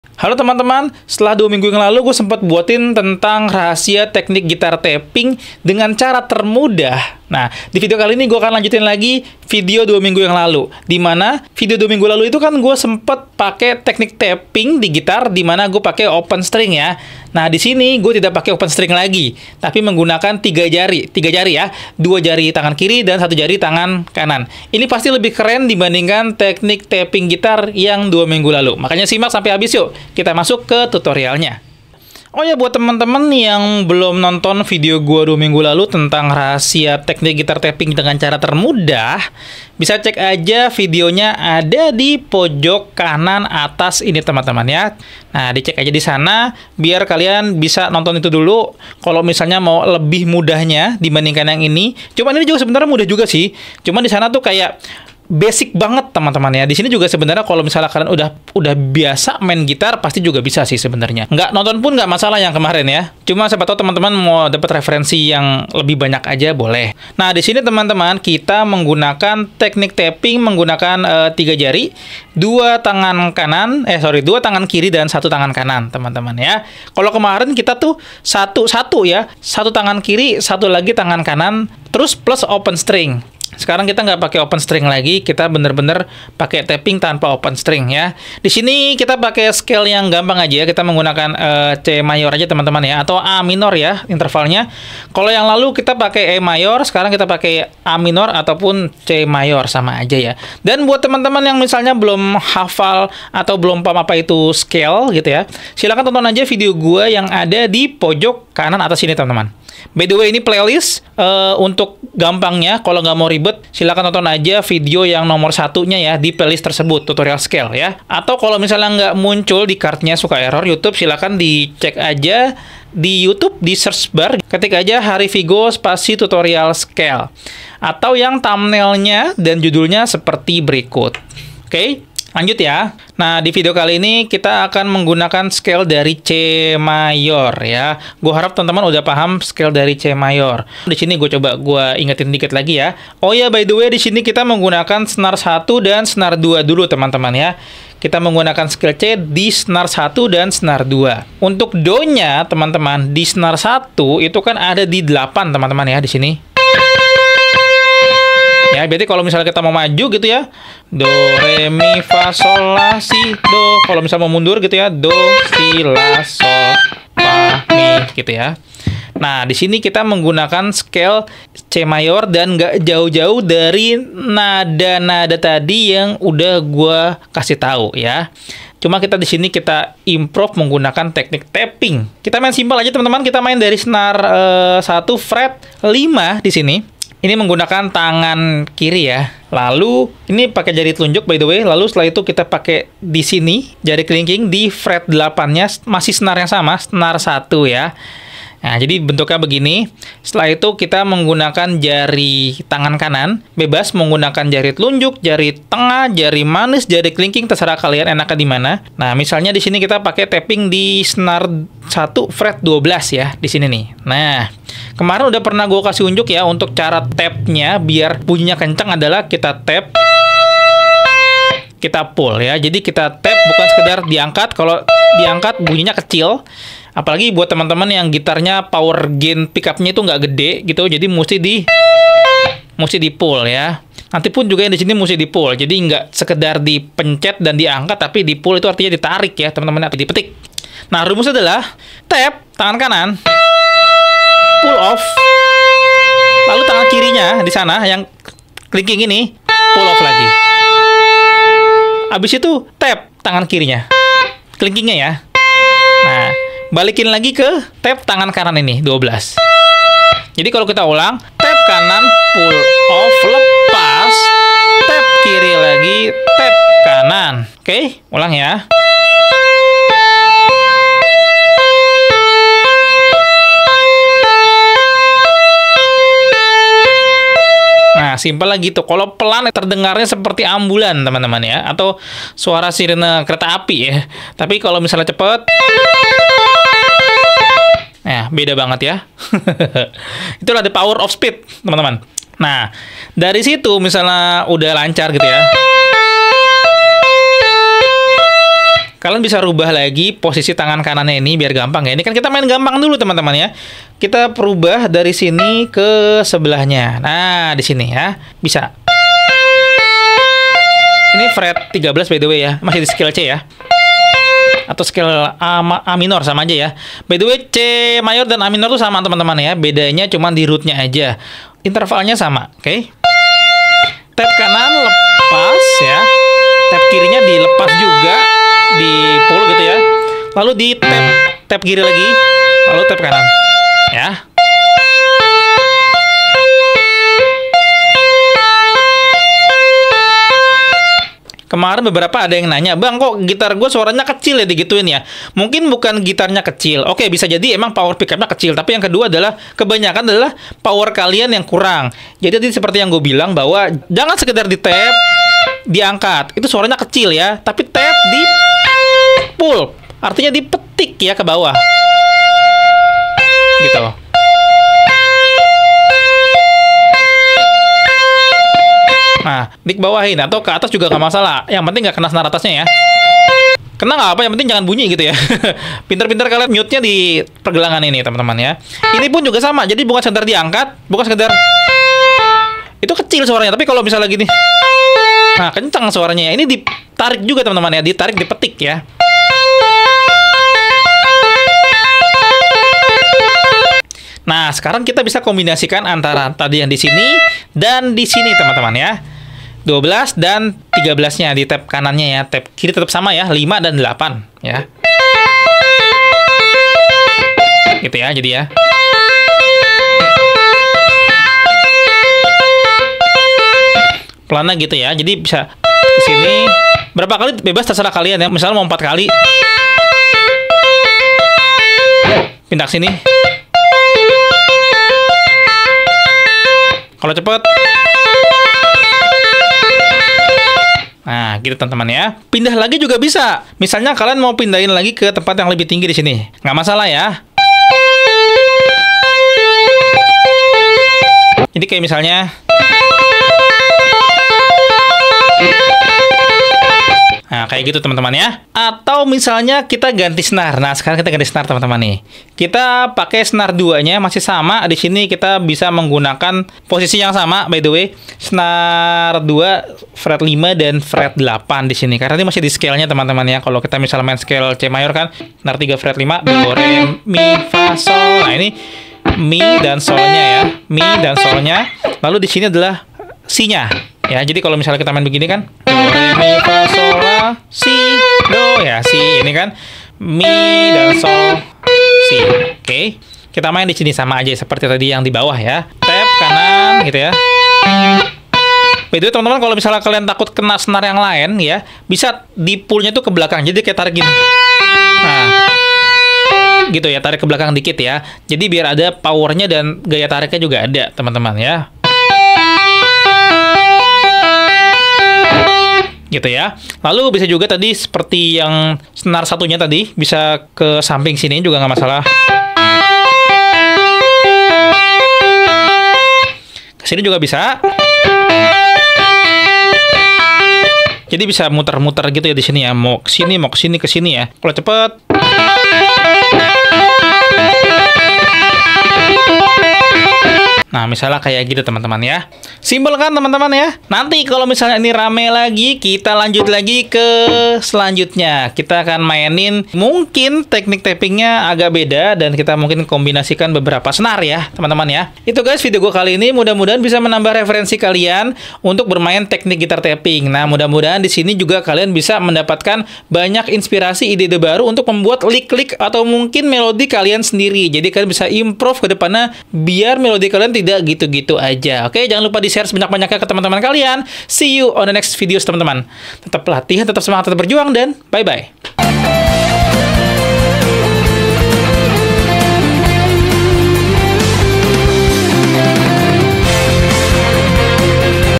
Rahasia Teknik Tapping Gitar Keren Untuk Pemula - Cuma Butuh 10 Menit
Melanjutkan video teknik gitar tapping 2 minggu lalu, kali ini gue buatin lagi rahasia teknik tapping gitar  keren yang bisa kalian kuasai dalam waktu 10 menit aja. Rahasia teknik tapping gitar kali ini gue jamin lebih keren dari 2minggu lalu. Nanitnya kalian bisa menambahkan teknik gitar ini untuk pengembangan improvisasi melodi gitar kalian!